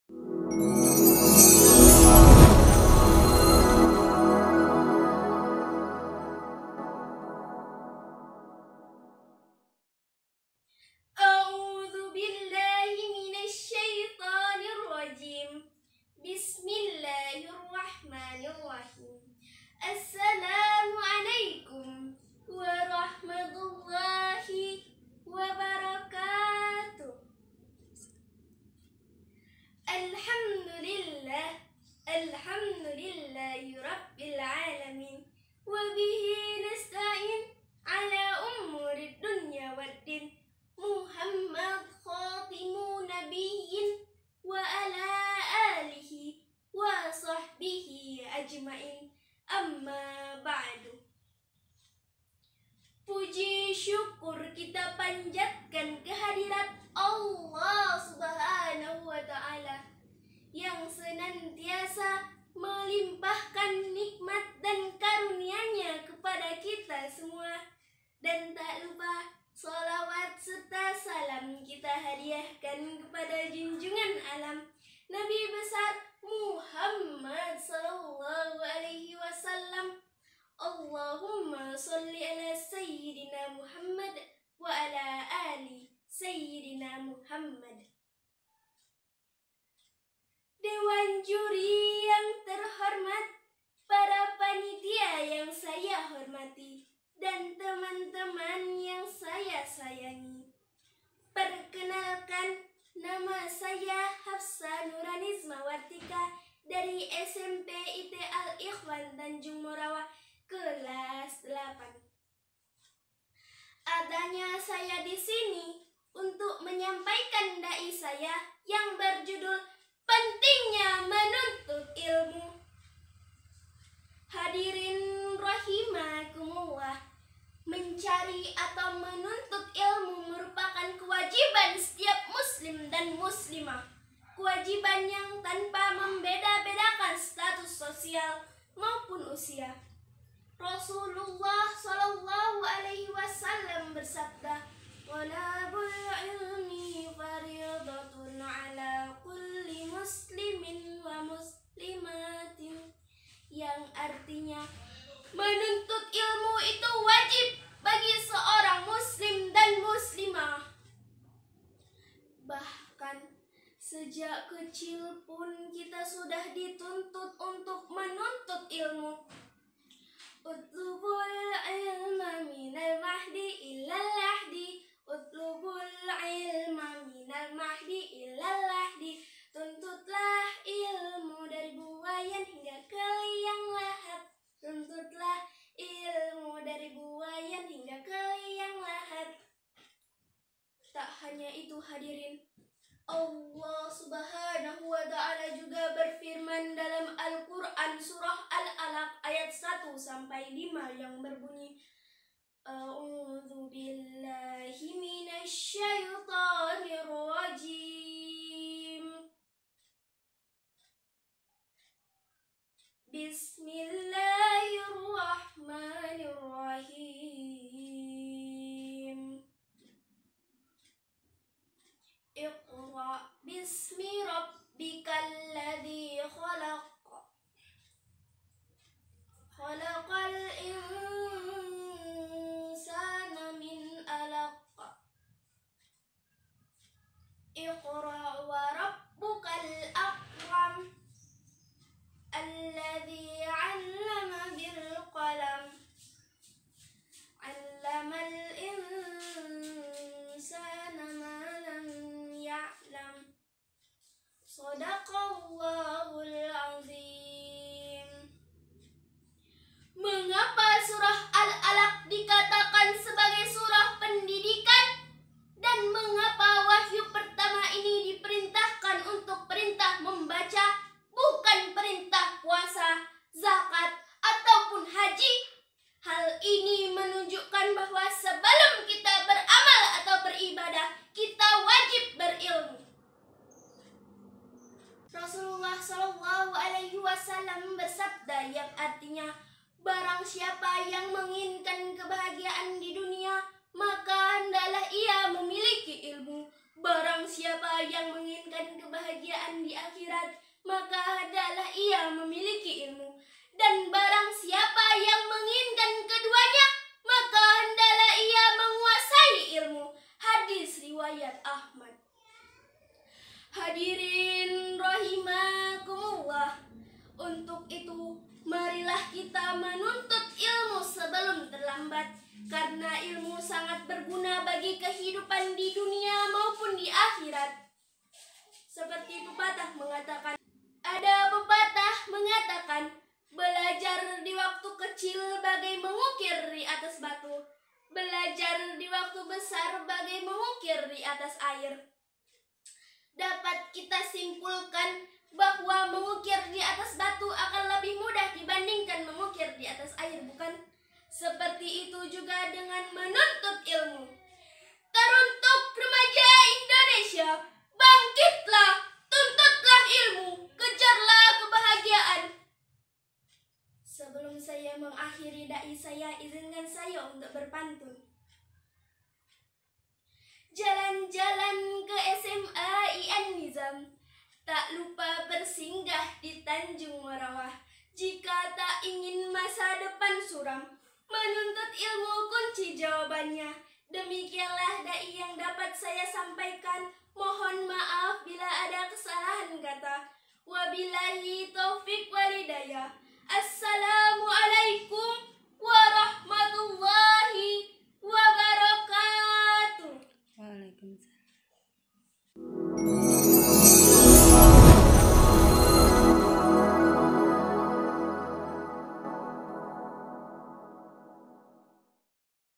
ASAFI ANNIZAM MEDAN 2020 | DA'I - Hafsa Nuranizma Wartika ( PENTINGNYA MENUNTUT ILMU )
ASAFI (An-Nizam Sport and Art Festival Of Islam) 2020 SMAI AN-NIZAM MEDAN

Nama Peserta : FATIH ALFANY SHODIQ
NPP                   : 06
Delegasi            : SMP IT AL IKHWAN T. MORAWA
Cabang Lomba : DAI

SMA AN-NIZAM MEDAN
Alamat : Jln. Tuba II/Perjuangan No 62 Medan Denai, Sumatera Utara